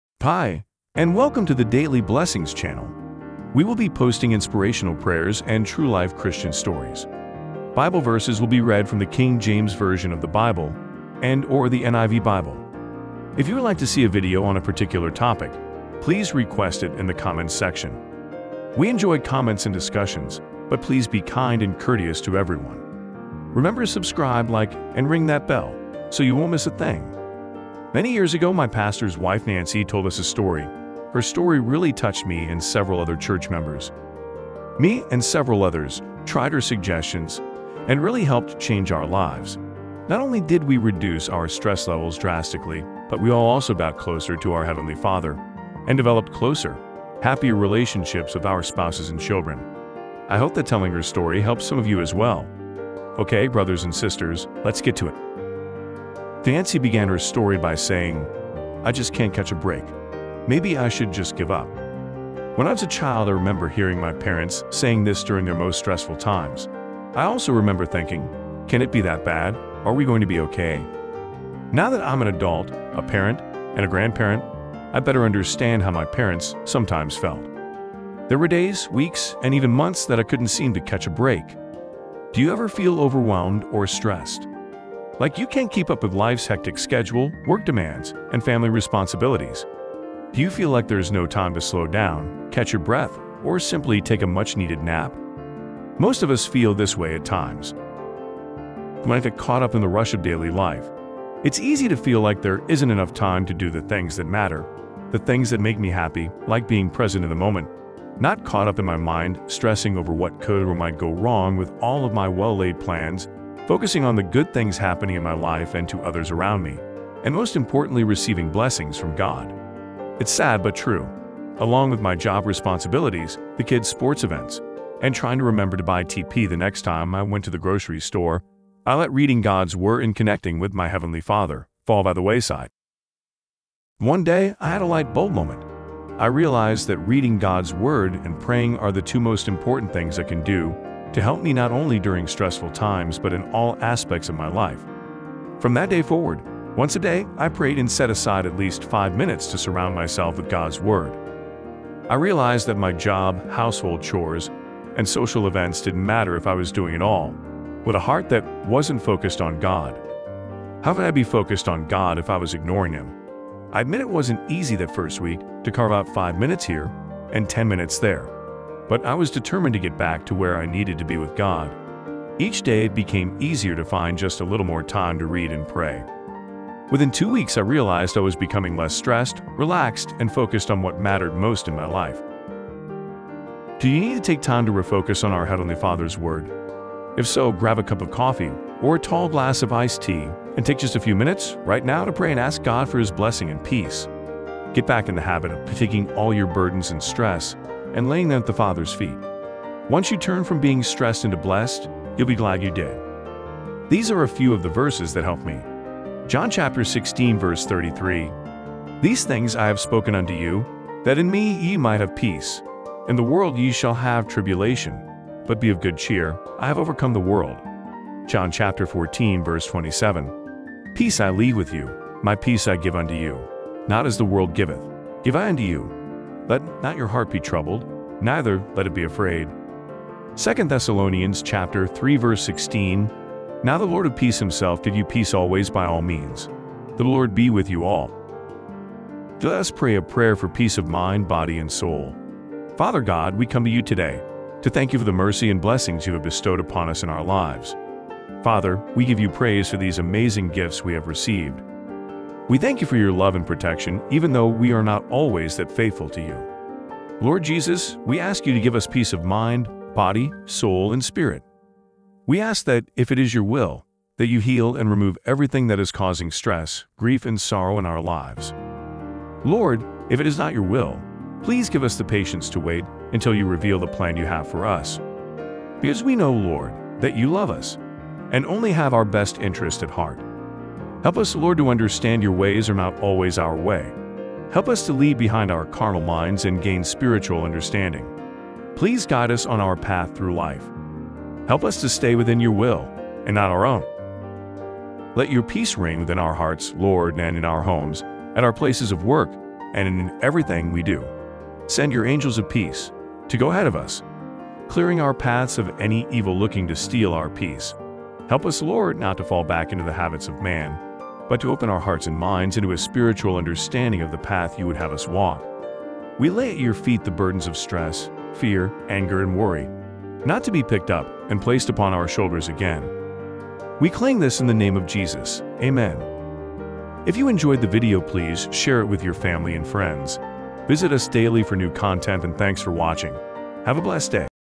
Bible Verses for Anxiety and Depression | Daily Blessings (Christian Inspirational Motivational )
During trying times it is easy to get stressed. Here are a few of my favorite bible verses and scriptures to help you to go from Stressed to Blessed.
Quotes are from the King James Version (KJV) and New International Version (NIV) bibles.

If you would like a video on a topic we haven't covered yet, post it in the comment section.

Bible Verses for Anxiety and Depression | Daily Blessings